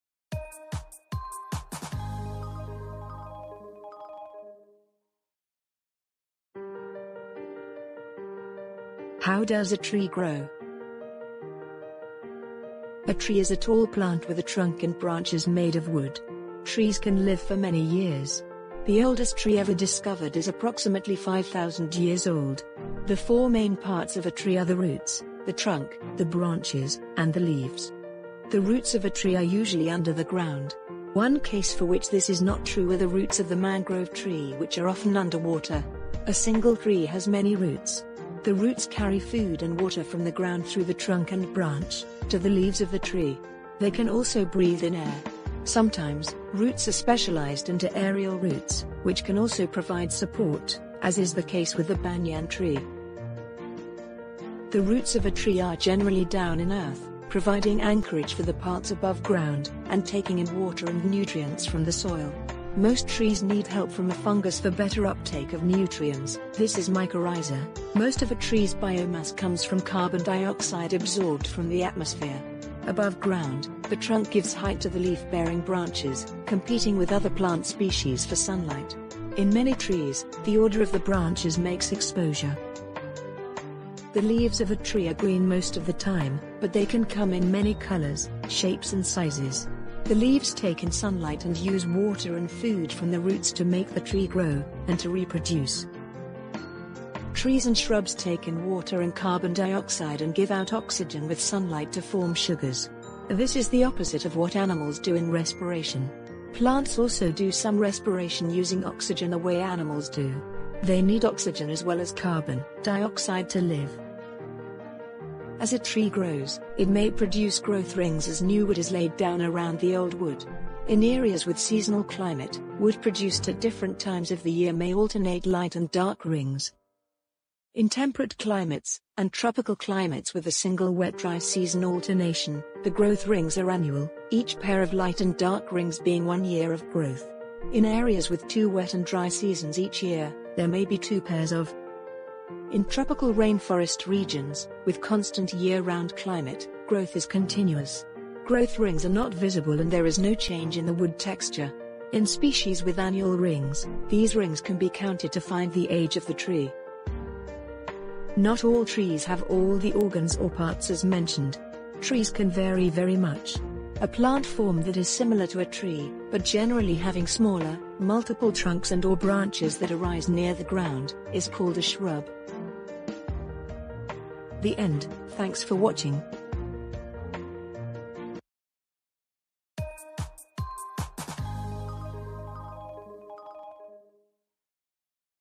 How Does a Tree Grow? By ECDHUB
"Hey there, curious young minds! Have you ever wondered, 'How Does a Tree Grow?' Well, let's embark on an exciting adventure to uncover the magical secrets of tree growth. You see, trees are like nature's skyscrapers, starting from tiny seeds and reaching for the sky. It all begins with a little seed planted in the soil. The seed snuggles into the earth, gets cozy, and starts to drink up water and soak in sunlight. Just like you need food and water to grow, trees need these elements too! As time passes, something incredible happens. The seed sprouts, sending out a tiny shoot, and that's the first step in answering our question, 'How Does a Tree Grow?' It's like nature's own birthday party! Slowly, the shoot grows taller and sprouts leaves, absorbing even more sunlight. With each passing day, the tree grows bigger and stronger. How Does a Tree Grow, you ask? Well, it's a lot like a magic show that happens right before our eyes in slow motion. And as the years go by, the tree becomes a home for birds, a playground for squirrels, and a shade-giver on hot summer days. So, remember, the next time you see a tree, think about 'How Does a Tree Grow?' It's a nature's masterpiece that all starts with a tiny seed and a little bit of magic."